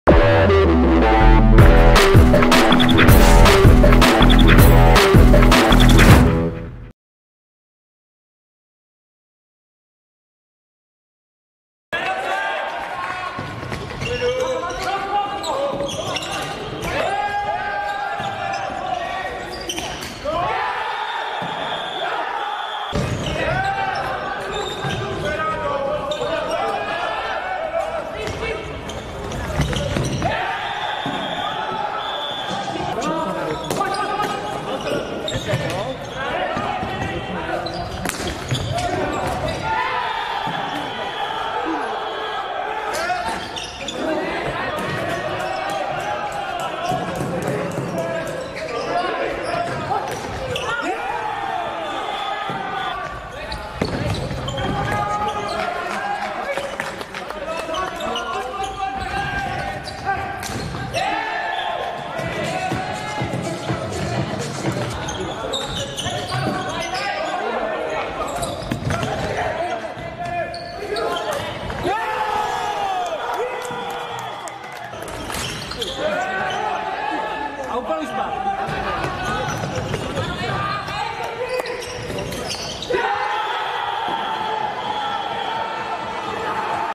7. kolo | FBC 4CLEAN Česká Lípa - SOKOLI Pardubice 3:4
branky: 22. Tomáš Novák, 26. Jakub Jirsa, 39. Radek Krajcigr - 5. Jan Savický, 8. Adam Šmíd, 20. Adam Šmíd, 53. Marcel Lehký; rozhodčí: Jiří Dolanský - Vojtěch Sýkora; vyloučení: 1:0; využití: 0:0; zásahy brankářů: Jan Dvořák 17 - Martin Haleš 19; po třetinách: 0:3, 3:0, 0:1;